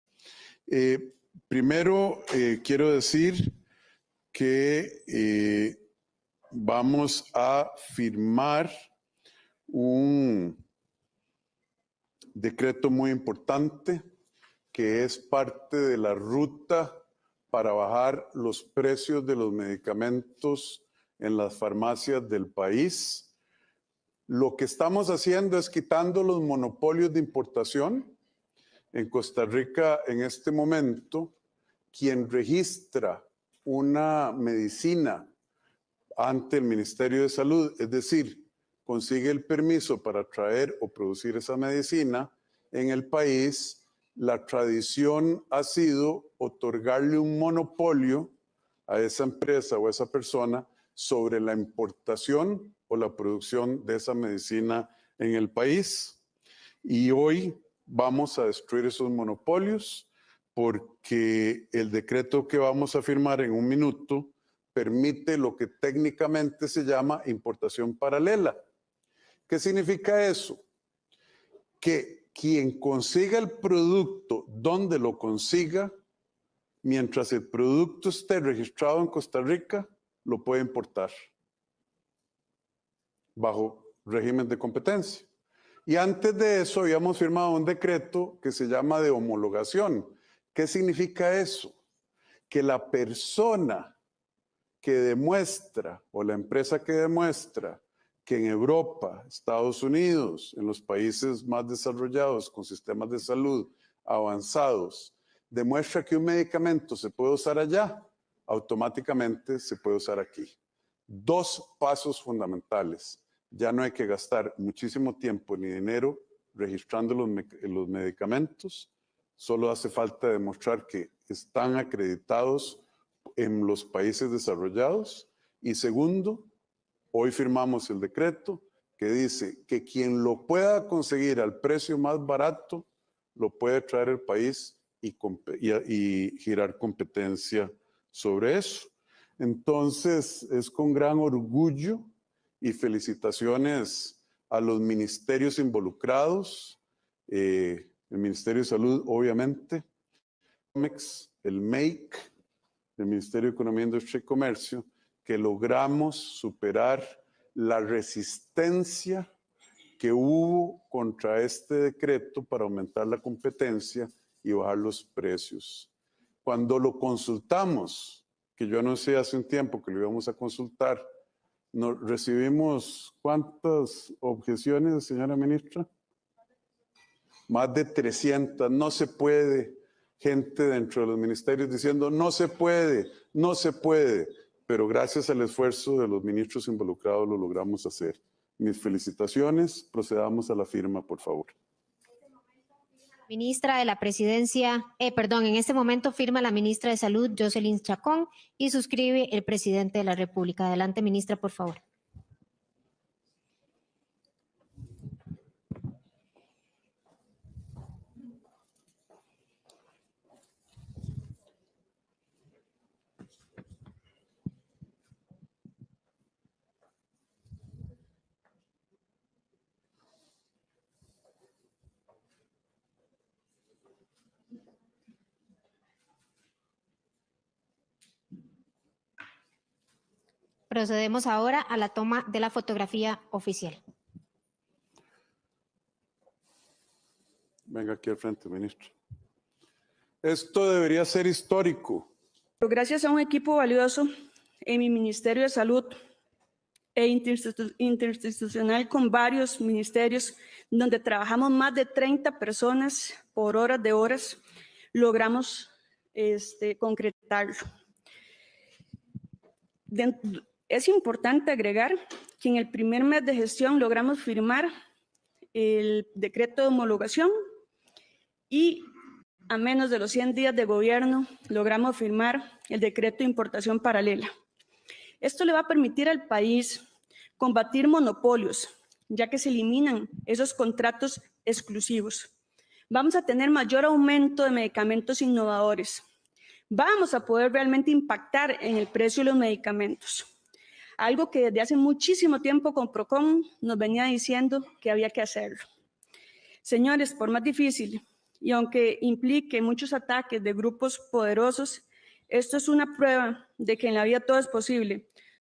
Decreto para bajar los precios de medicamentos se firmó hace casi un año
Se trató del segundo decreto ejecutivo que firmó el actual gobierno para intentar cumplir la promesa de campaña electoral que hizo el hoy mandatario Rodrigo Chaves Robles.